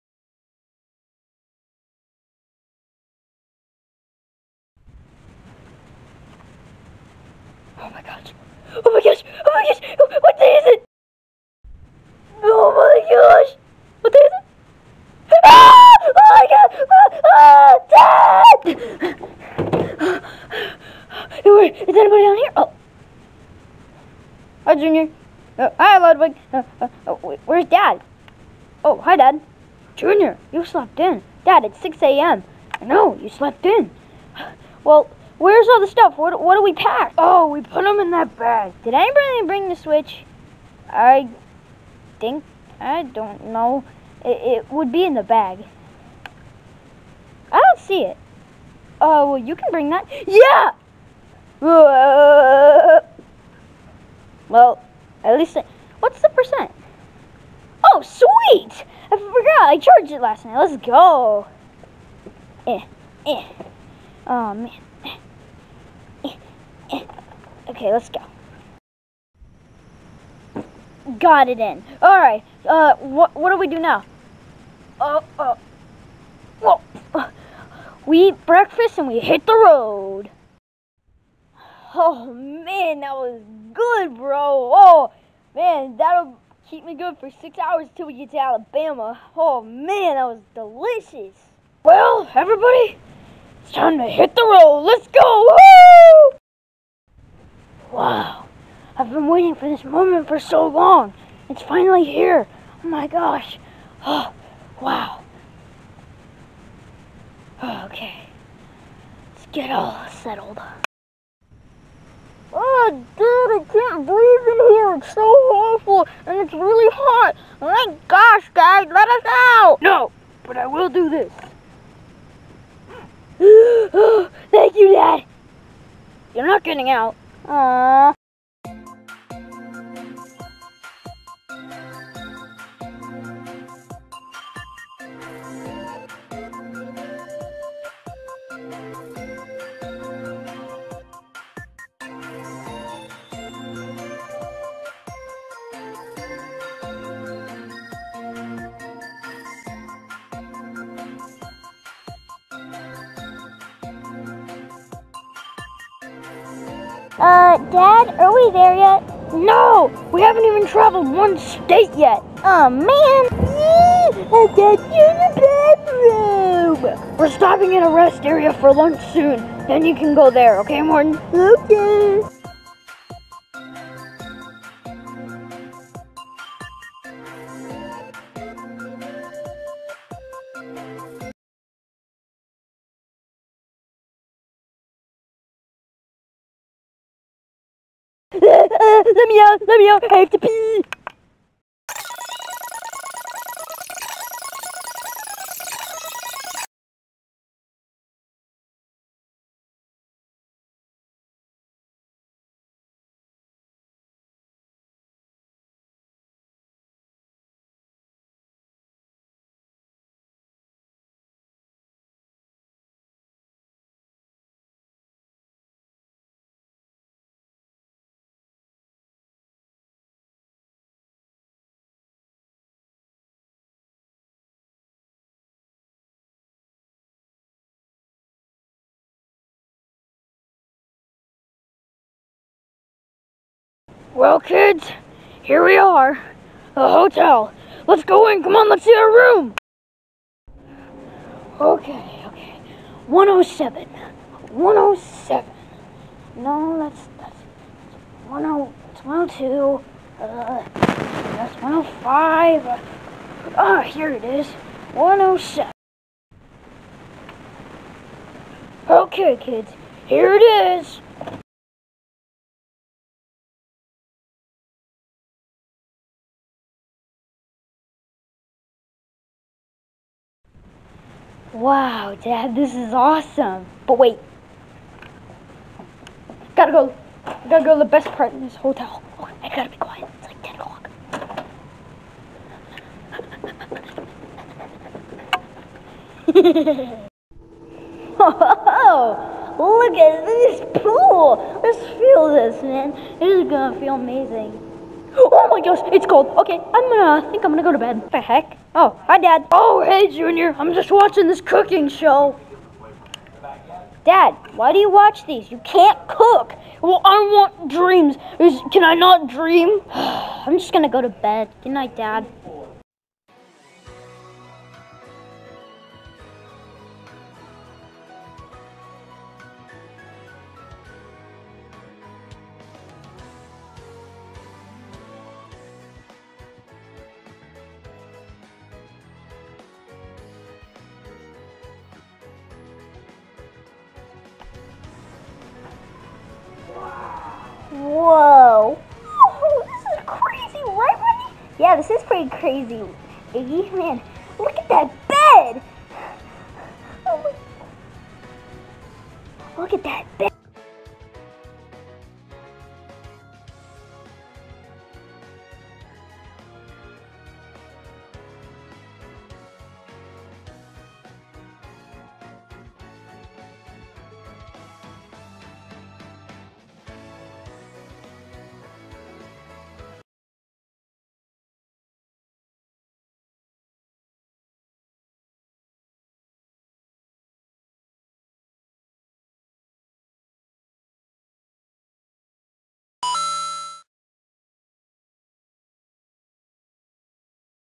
Koopalings go to Panama city! Episode 1 on the road
Bro This Vacation was awesome! I tried my best to make videos but I also Spent most of the time with My family! This series will be going into fall like Hopefully This will be done by October. Thanks for tuning in I am going to work on episode 2. See you then!